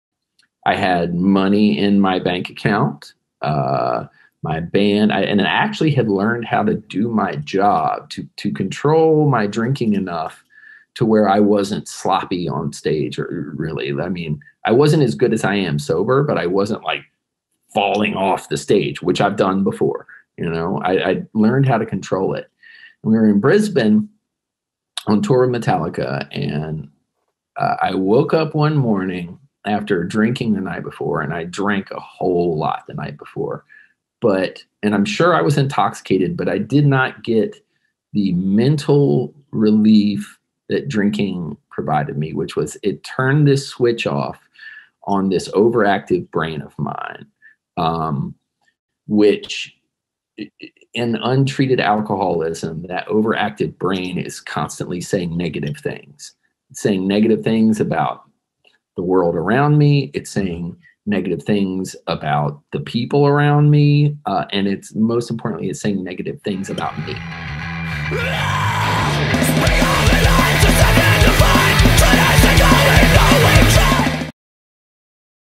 Randy Blythe Talks Quitting Drinking And Using Alcohol It As A Crutch To Avoid Negative Thoughts
Here's a clip from Series 1 Episode 8 featuring guest Randy Blythe (Lamb of God) and your hosts Jesse Leach (Killswitch Engage, Times of Grace) and Matt Stocks (Life in The Stocks).

In this clip, Randy Blythe talks waking up after a night of hard drinking and finally feeling like he wasn't getting the relief alcohol used to give him.  It would shut off his over active brain running on with negative thoughts, but finally that vice was no longer working.  Drop us an email or comment and let us know your story!

Produced by:
Too Much Content
toomuchcontent.live